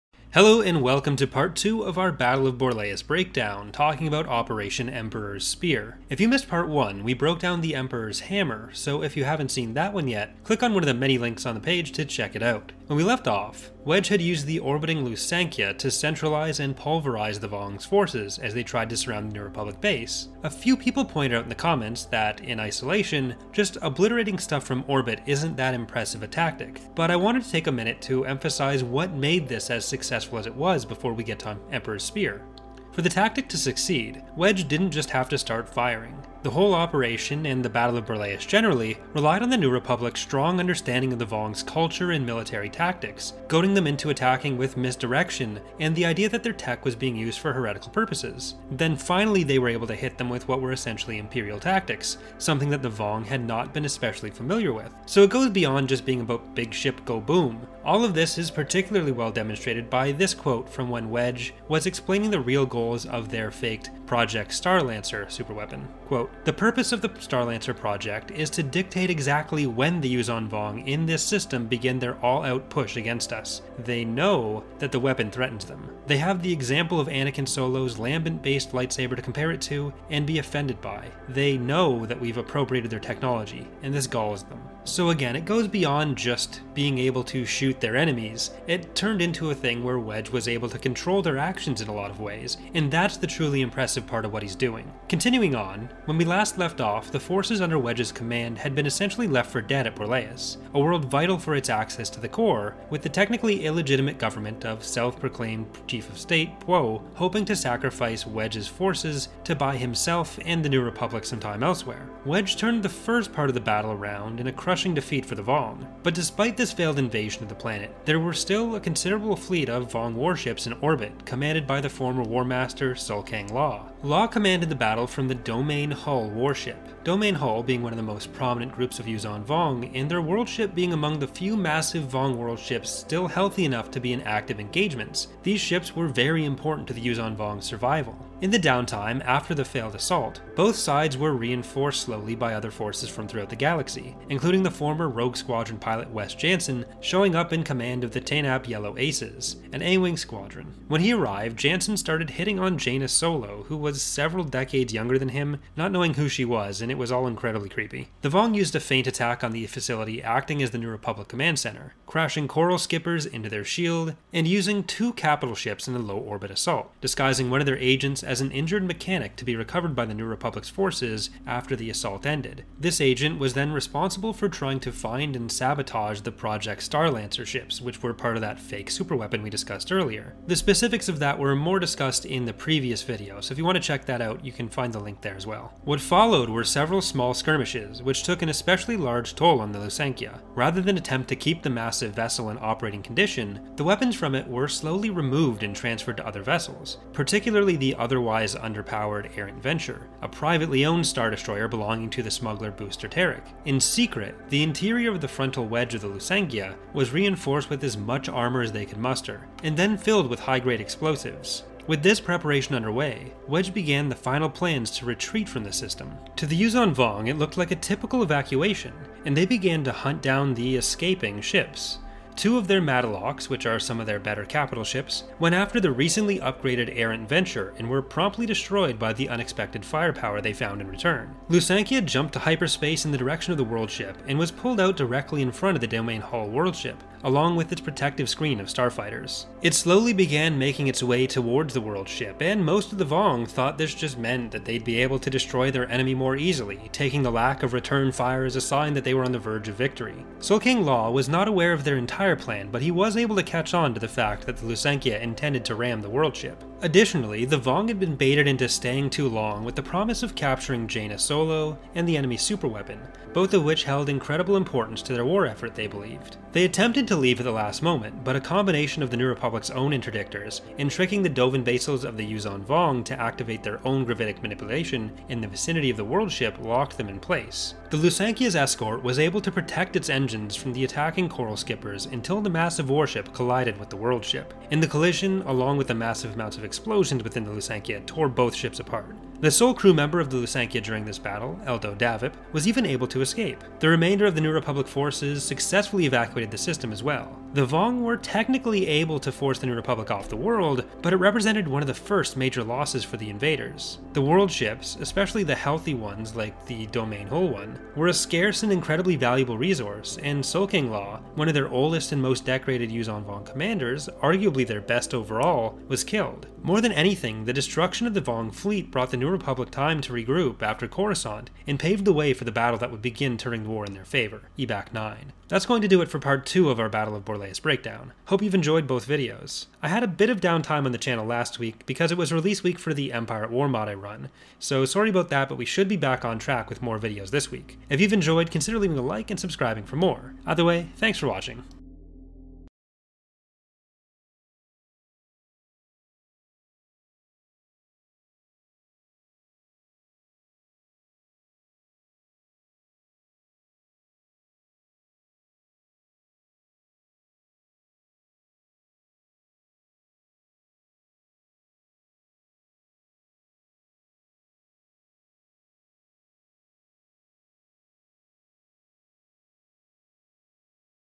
Battle Breakdown: Super Star Destroyer Ramming Tactics! | Borleias Part 2
How Wedge took back Bakura using a super star destroyer.